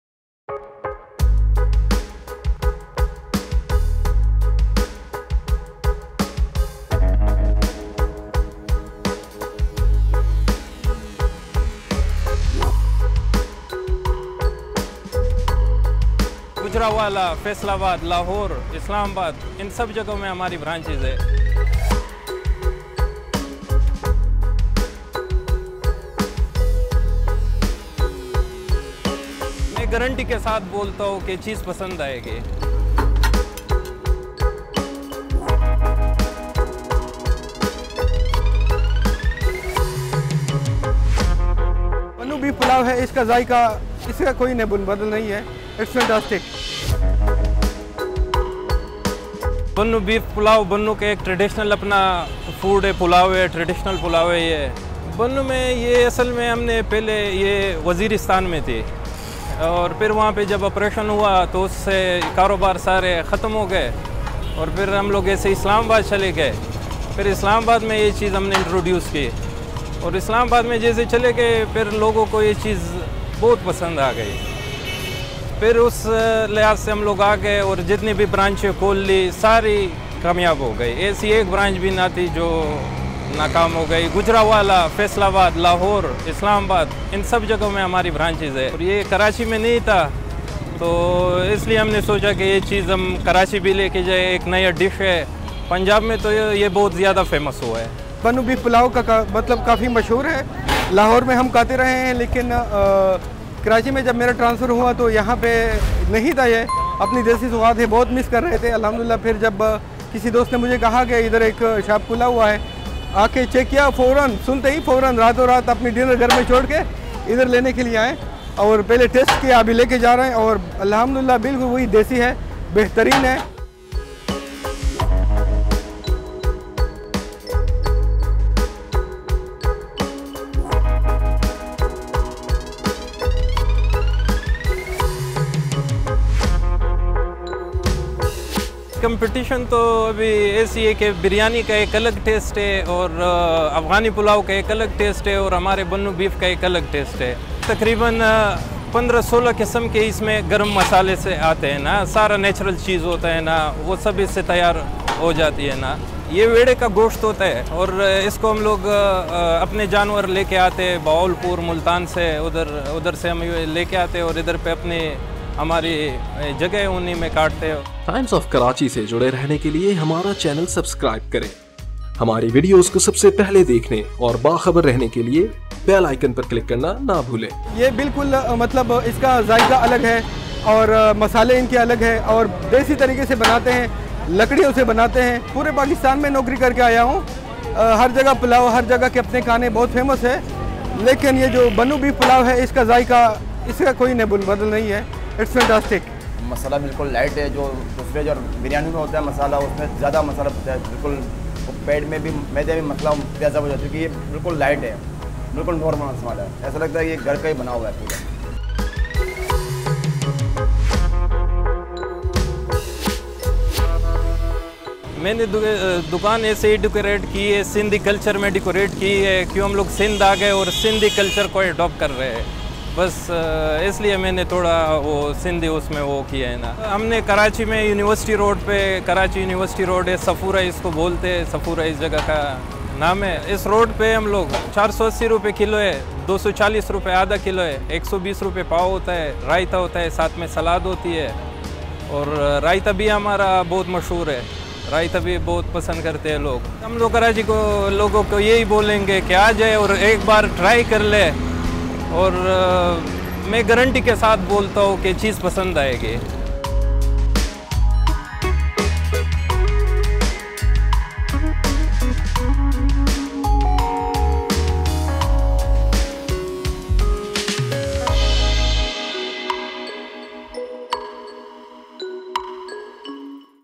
Bannu ki mashoor beef pulao Punjab k baad Karachi main bhi
After several cities in Pubjab, Bannu's popular beef pulao is now available at Sofora on University Road in Karachi.